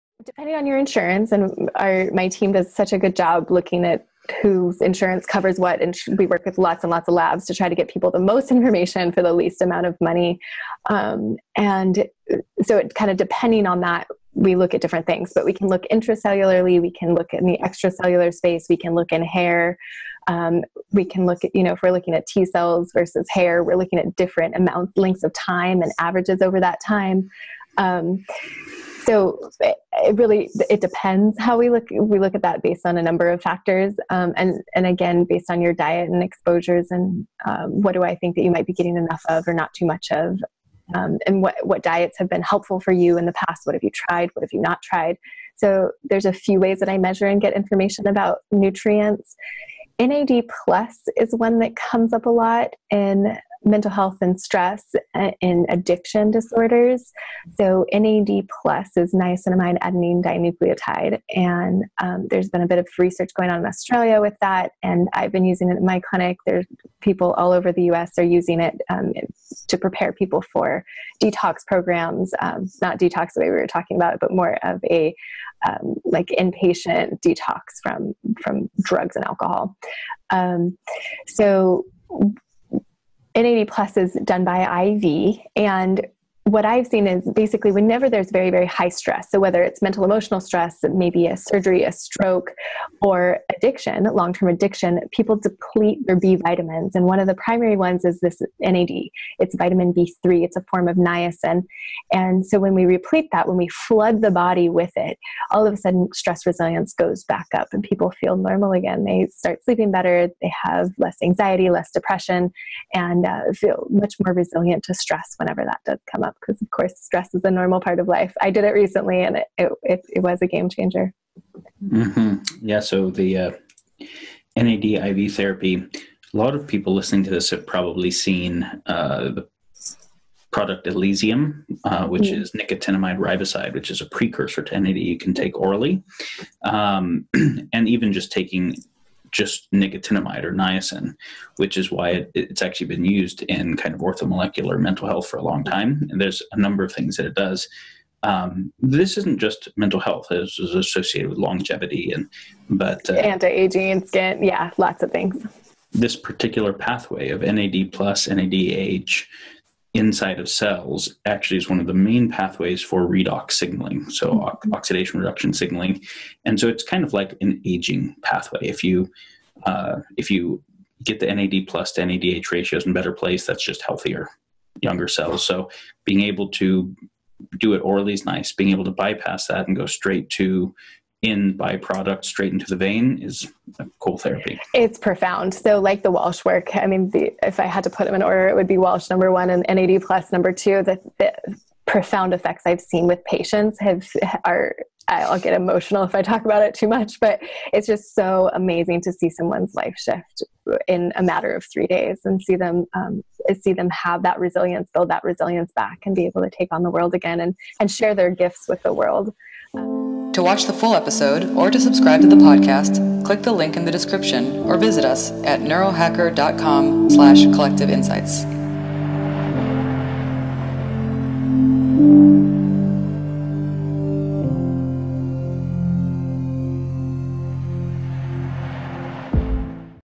Understanding NAD+ Therapy with Dr. Heather Sandison
There are many factors which can impact your physical and mental health, from diet to toxin exposure to genetic composition. Of the many therapies available, one has had a significant effect on reducing stress and even treating addiction: NAD+. Dr. Sandison discusses what NAD IV therapy is and how it can help people in the detox process by making them more resilient to stress.